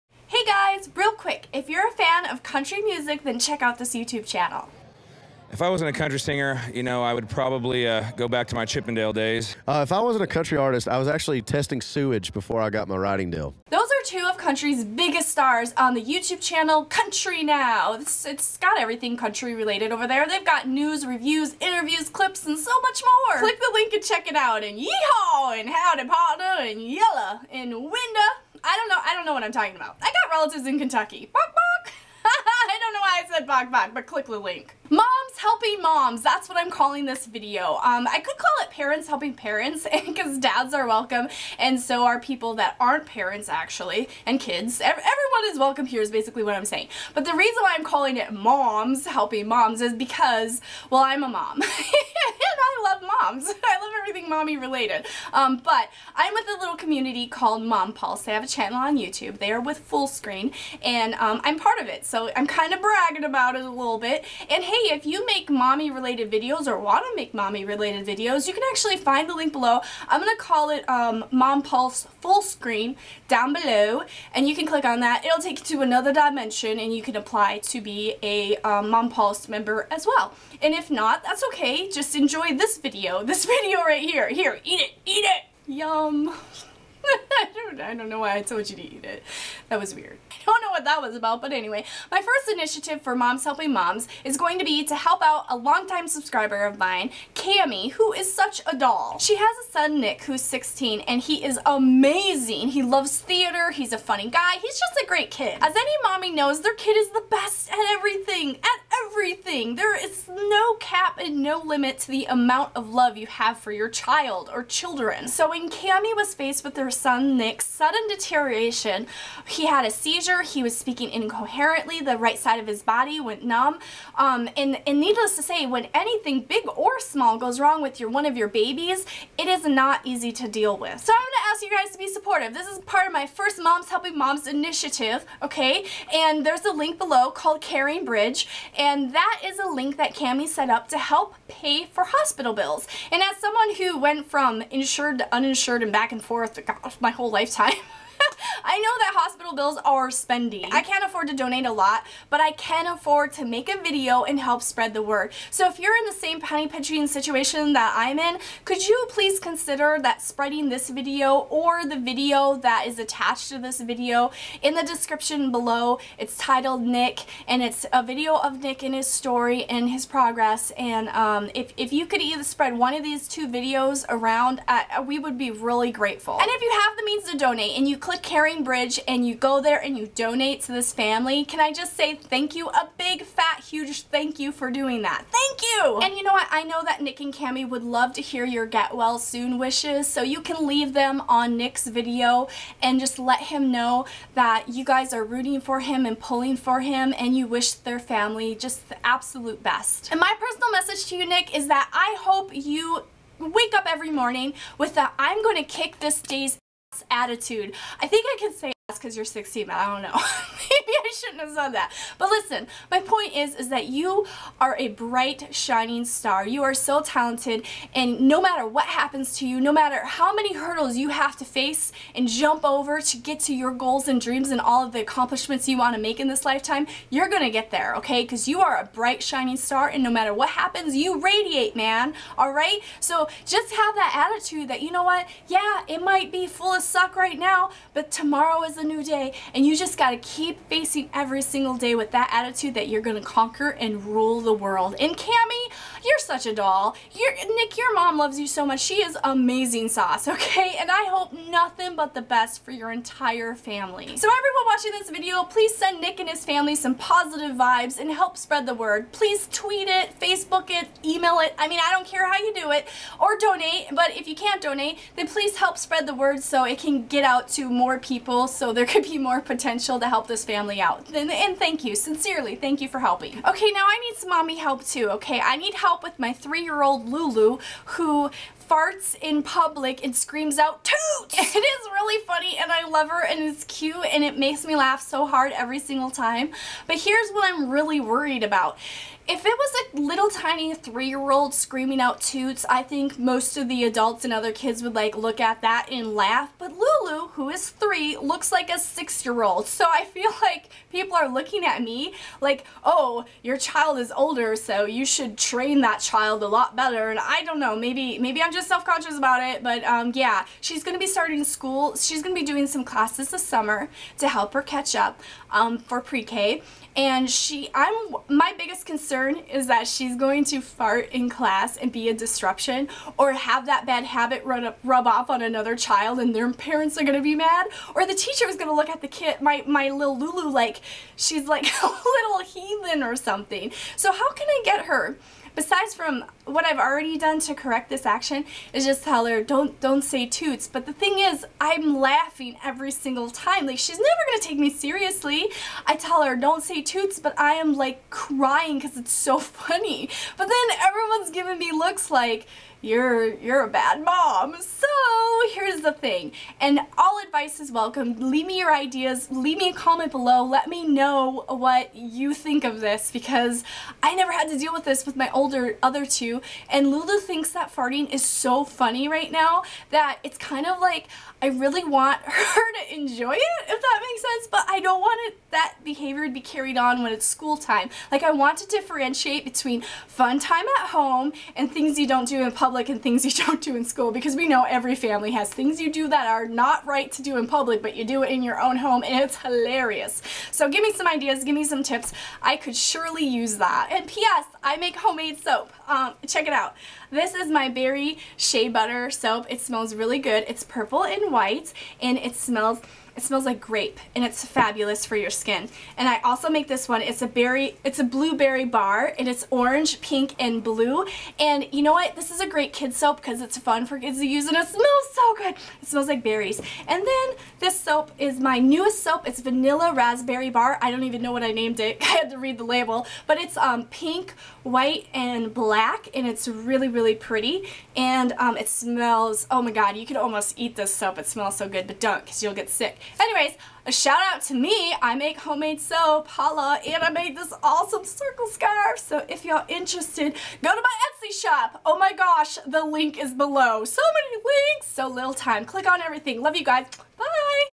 Moms Helping Moms
This video is about Moms Helping Moms and establishing a great fun community of parents who can give great advice and ask for help when they need it!

SOAP! Etsy Shop